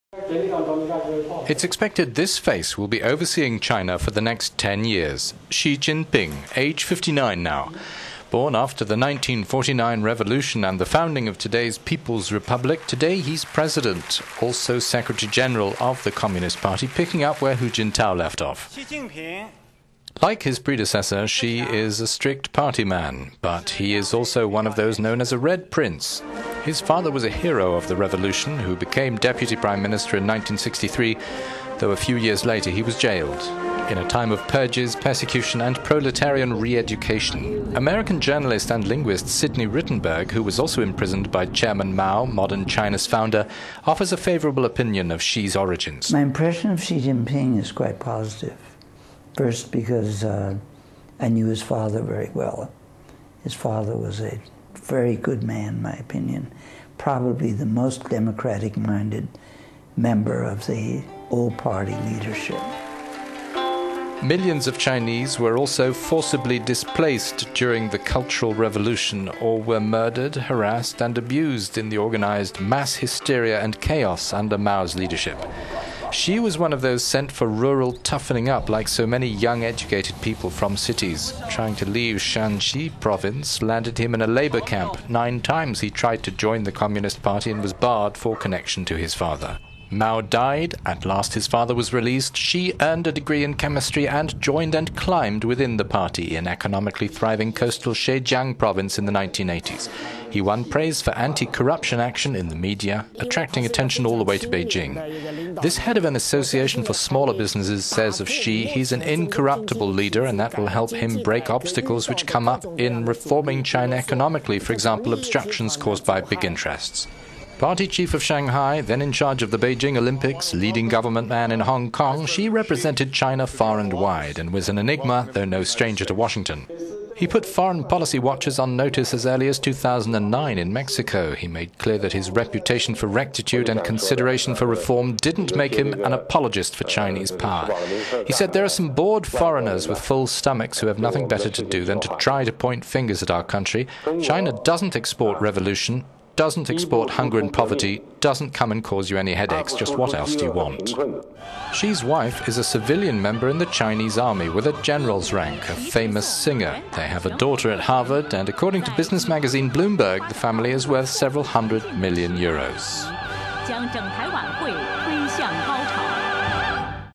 Xi head man of China's future
It is expected that the face which will be overseeing China for the next decade will be that of Xi Jinping, now age 59. Born after the 1949 revolution and the founding of today's People's Republic, today he is President, also Secretary General of the Communist Party, picking up where Hu Jintao left off.

Like his predecessor, Xi is a strict party man. But he is also one of those known as a 'Red Prince'. His father was a hero of the revolution, who became deputy prime minister in 1963, though a few years later he was jailed - in a time of purges, persecution and proletarian re-education.

American journalist and linguist Sidney Rittenberg, who was also imprisoned by Chairman Mao, the founder of modern China, offered a favourable opinion of Xi's origins.

The former translator Rittenberg said: "My impression of Xi Jinping is quite positive. First, because I knew his father very well. His father was a very good man in my opinion, probably the most democratic-minded member of the old party leadership."

Millions of Chinese were also forcibly displaced during the subsequent 'Cultural Revolution', or were murdered, harassed and abused in the organised mass hysteria and chaos under Mao's leadership.

Xi was one of those sent for rural toughening up, like so many young, educated people from cities. Trying to leave Shaanxi province landed him in a labour camp. Nine times he tried to join the Communist Party and was barred for connection to his father.

Mao died, at last his father was released, Xi earned a degree in chemistry and joined and climbed within the party, in economically thriving coastal Zhejiang province in the 1980s.

He won praise for anti-corruption action, in the media and attracting attention all the way to Beijing.

Zhou Dewen, the head of an association for smaller businesses in Wenzhou said of Xi: "He is an incorruptible leader, and that will help him break various obstacles in economically reforming China, for example, obstructions caused by big interests."

Party Chief of Shanghai, then in charge of the Beijing Olympics, leading government man in Hong Kong... Xi represented China far and wide, and was an enigma, though no stranger to the White House.

He put foreign policy watchers on notice as early as 2009, in Mexico. He made clear that his reputation for rectitude and consideration for reform did not make him an apologist for Chinese power.

Xi, by then Vice President of China, said: "There are some bored foreigners with full stomachs, who have nothing better to do than try to point fingers at our country. First, China does not export revolution; second, China doesn't export hunger and poverty; third, China doesn't come and cause you any headaches. Just what else do you want?"

Xi's wife Peng Liyuan is a civilian member in the Chinese army with a general's rank - a famous singer. They have a daughter at Harvard, and, according to business magazine Bloomberg, the family is worth several hundred million euros.